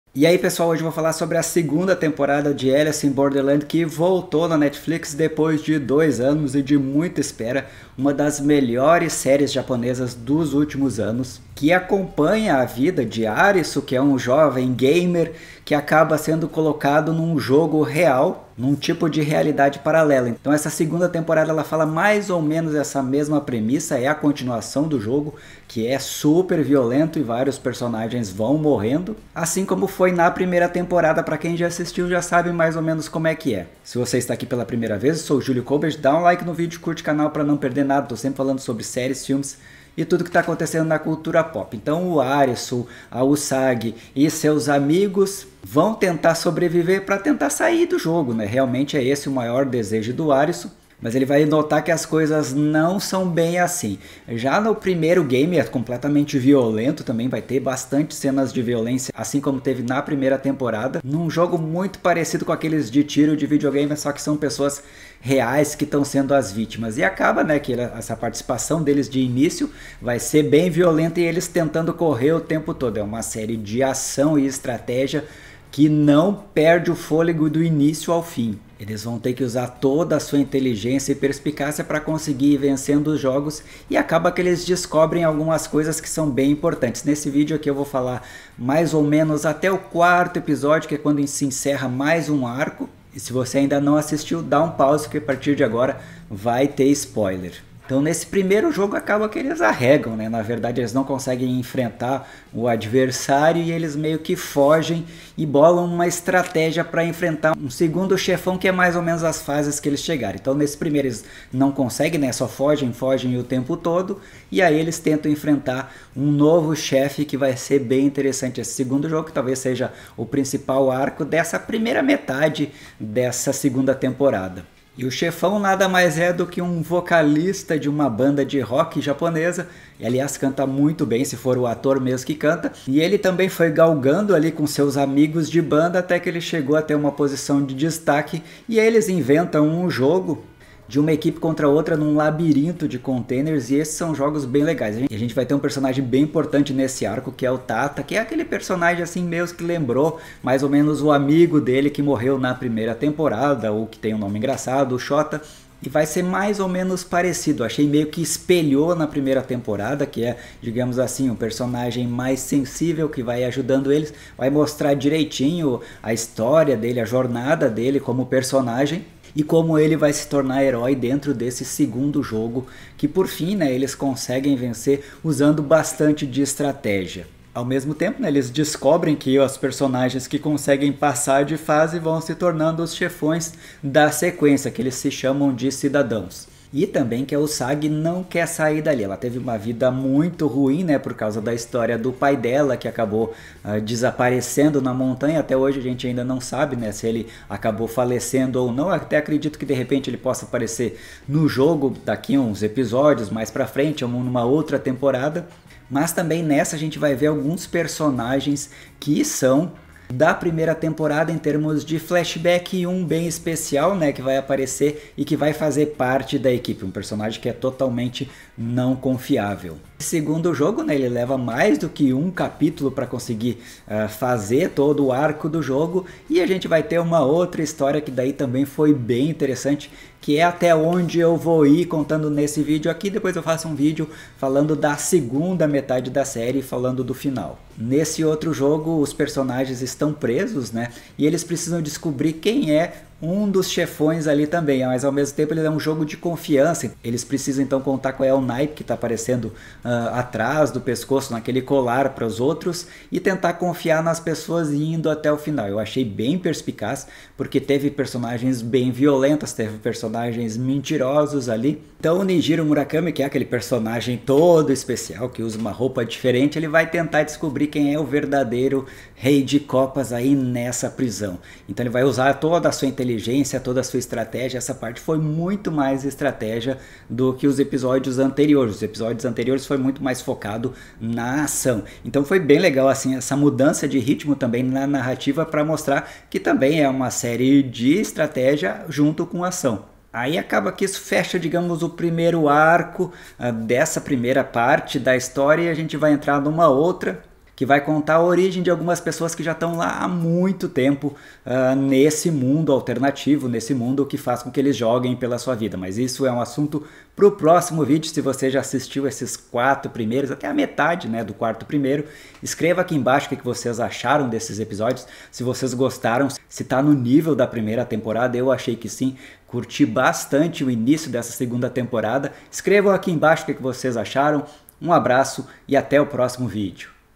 ALICE IN BORDERLAND 2ª TEMPORADA VOLTOU MUITO BOA - EPISÓDIO 1 AO 4 NETFLIX. CRÍTICA
Neste vídeo falo sobre os episódios 1, 2, 3 e 4 da segunda temporada de Alice in Borderland da Netflix que voltou muito bem. Uma das melhores séries japonesas dos últimos anos.